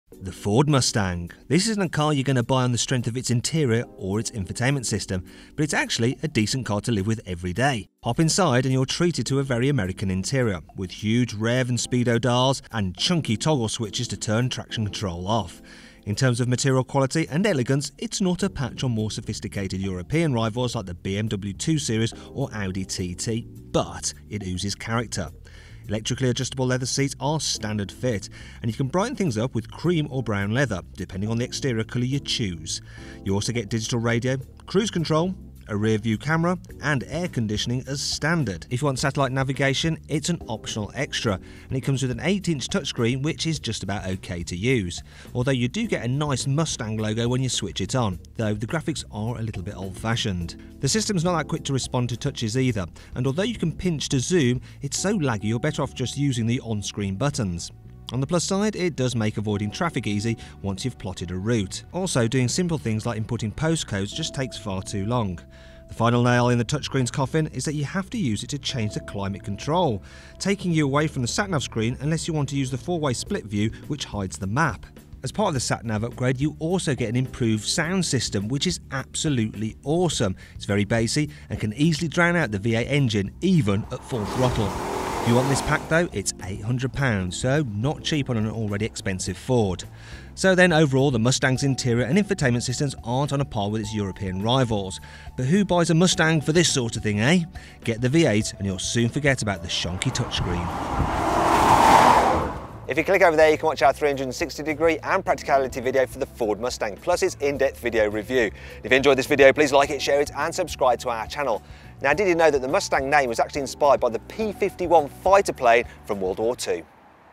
Ford Mustang V8 Sports Car 2018 infotainment and interior review | Mat Watson Reviews
We take an in-depth look at the Ford Mustang V8's infotainment system and interior.

Watch our Ford Mustang V8 vs BMW M235i vs Audi TT review